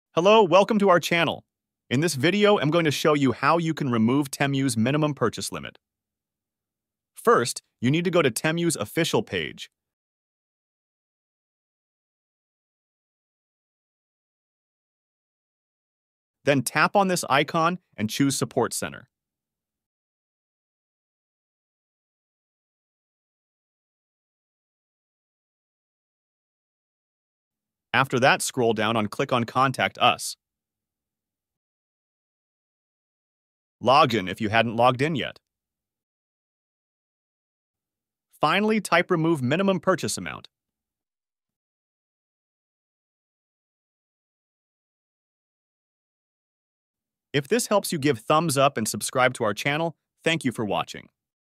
How to Remove Temu's Minimum Purchase Limit (2024)
In this video I'll show you How to Remove Temu's Minimum Purchase Limit (2024). The method is very simple and clearly described in the video. Follow all of the steps How to Remove Temu's Minimum Purchase Limit (2024)